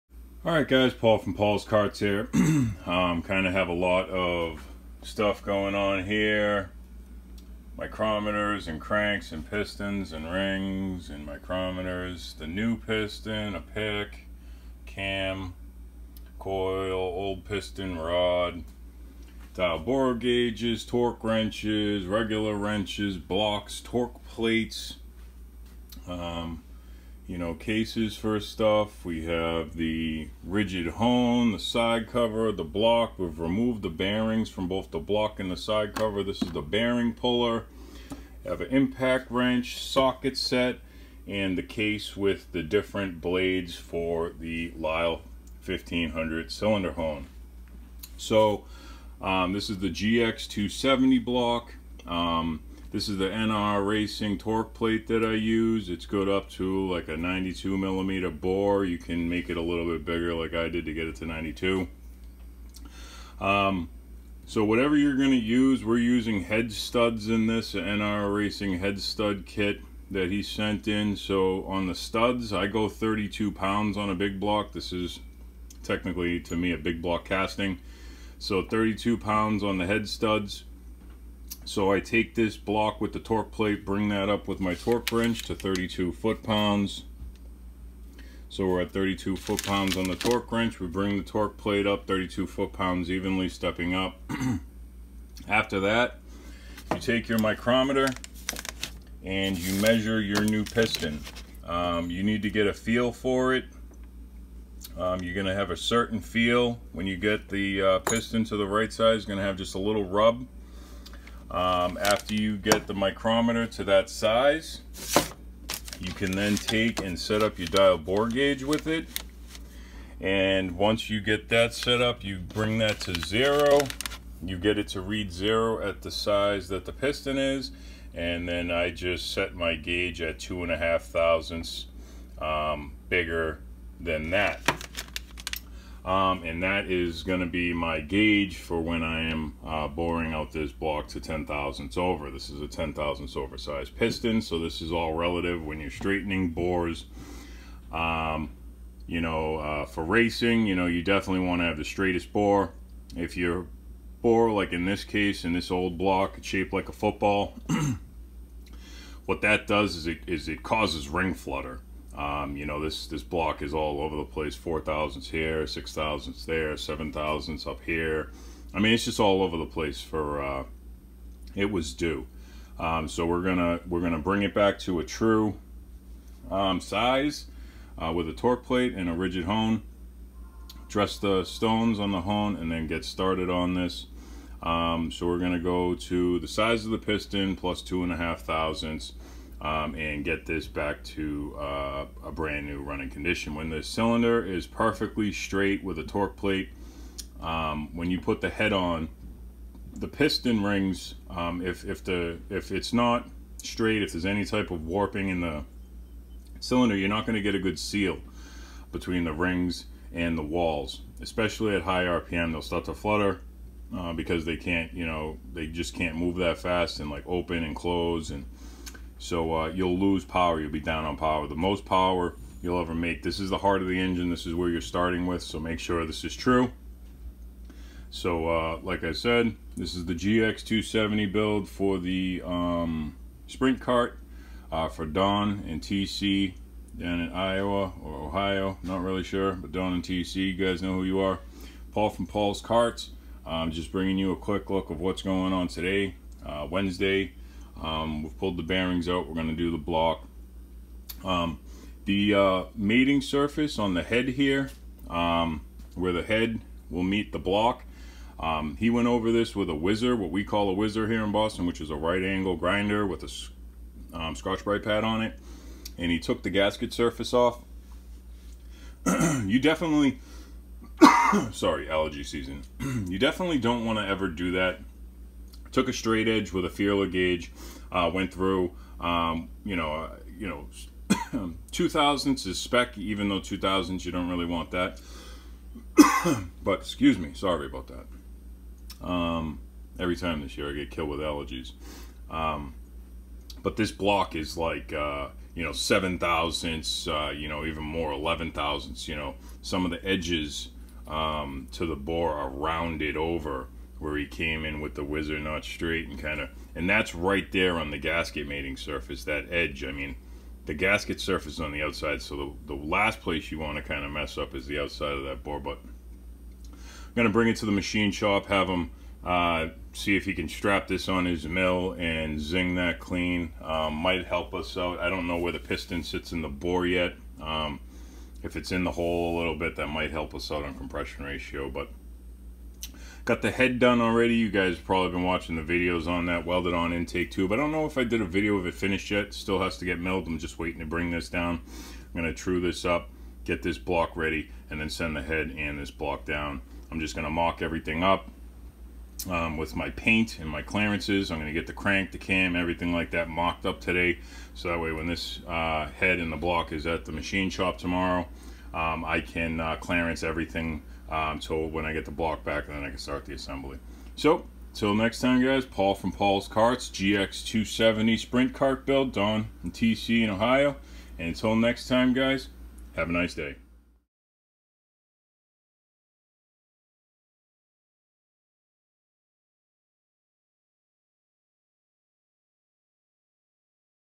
HONDA GX270 engine build ( over sized piston & bore straightening ) // part
The heart of the engine , the foundation , the tools needed and a guide to boring out a gx270 block